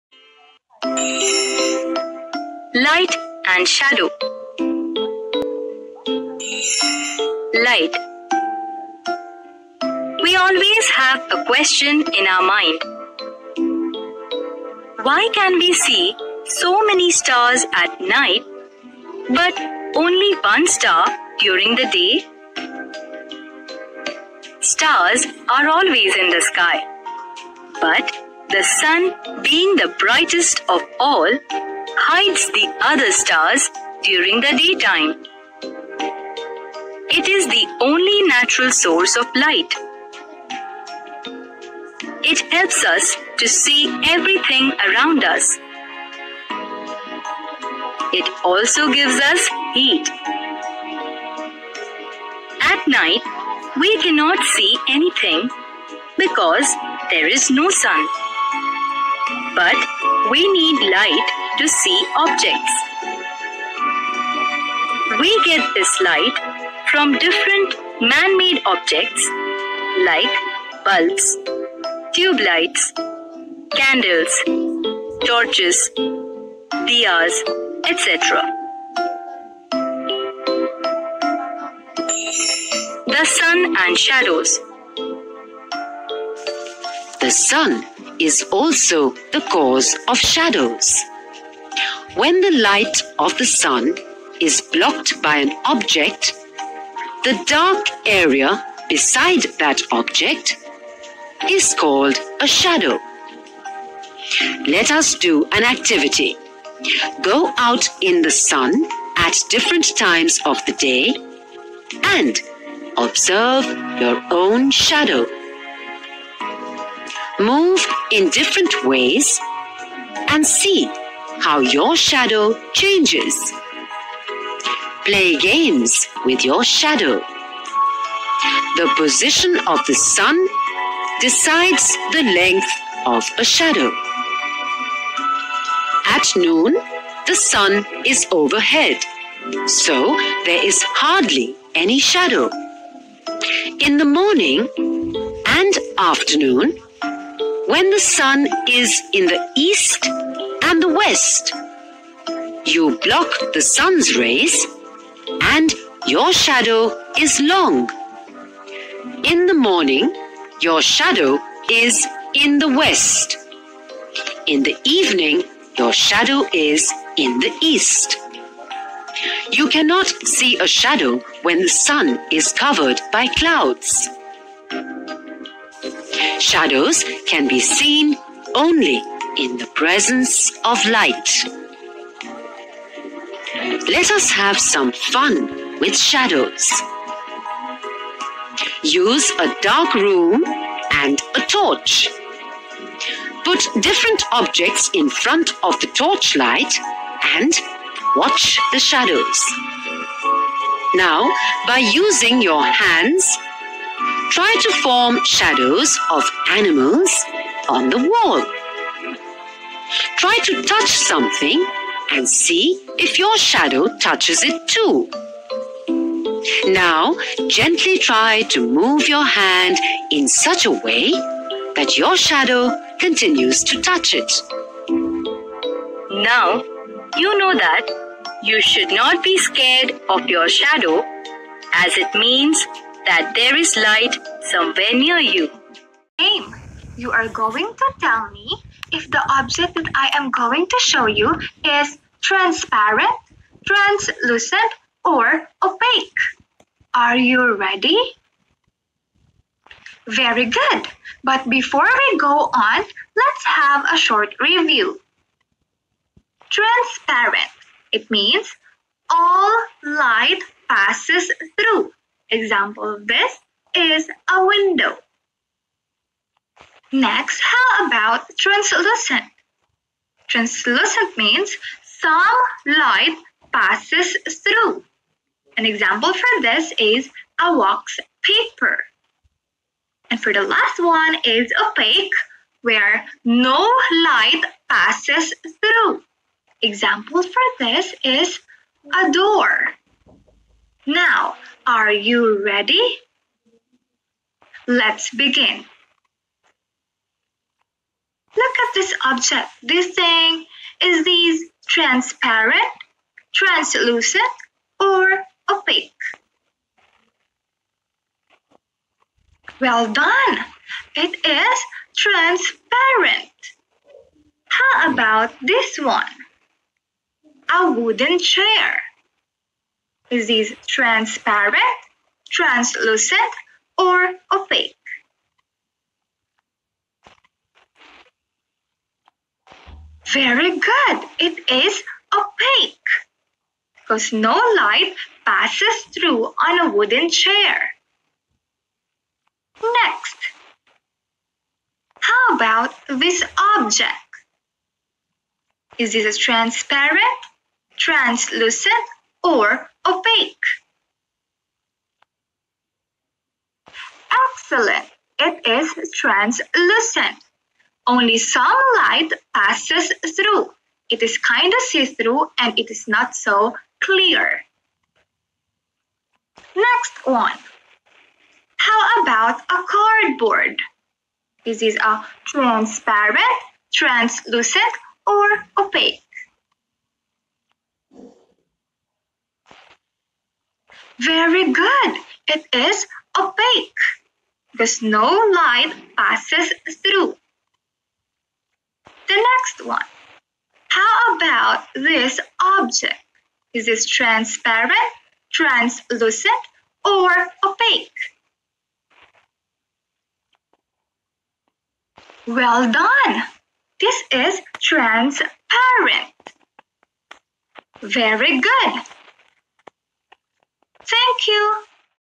Class # 22 of Science for Grade 2 (Light and Shadow)
AOA Hoping for your best of Health and Emaan.
Dear Students!
In this video we will learn what is light and shadow.

What is transparent.
What is Translucent.
What is Opaque.
Sources of light.
Natural source of light.
Artificial source of light.
sunshine and shadow.
Observe your own shadow.
Shadows of sun light.
Shadows of tubelight
Shadows of bulb.
Clouds.

Dare-Arqam-Schools